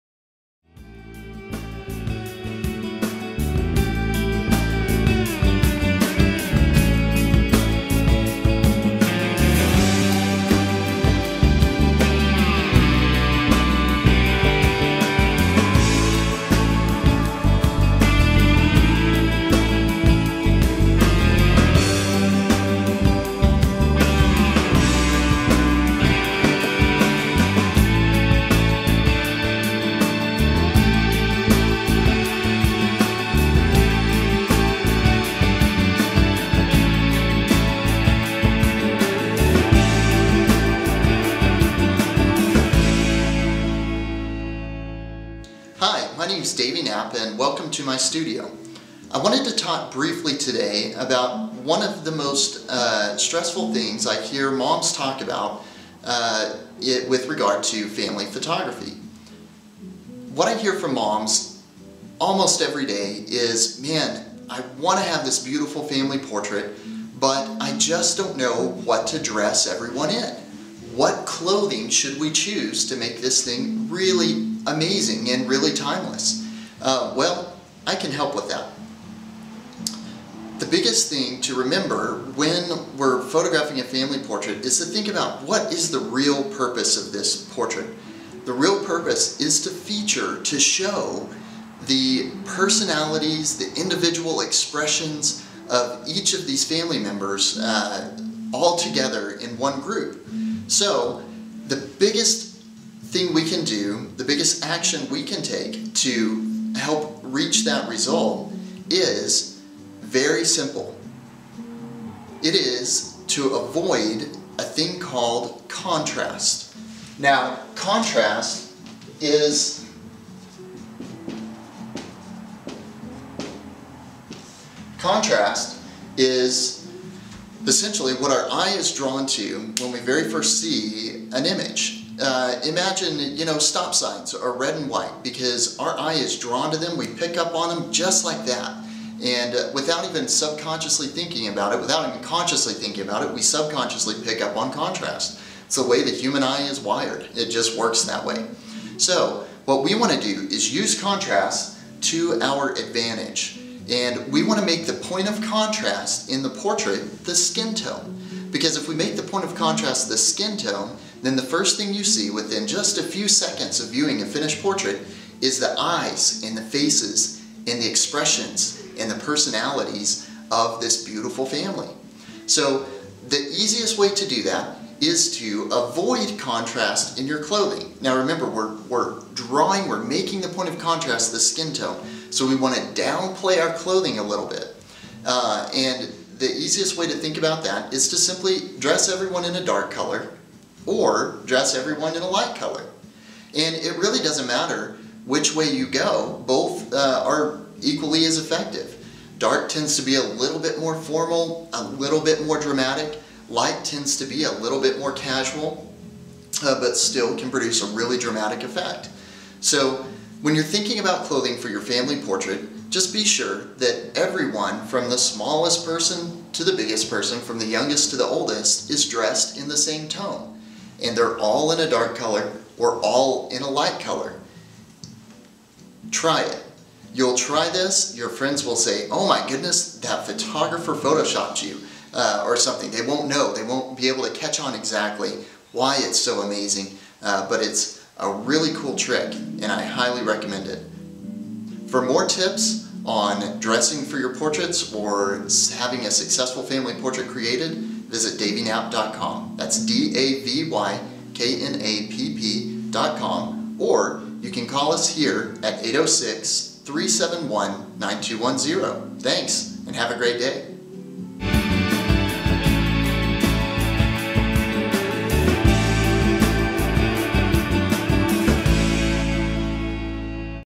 How To Dress Your Family For A Stunning Family Portrait
Photographer Davy Knapp talks about one of the most effective ways to dress for a family portrait.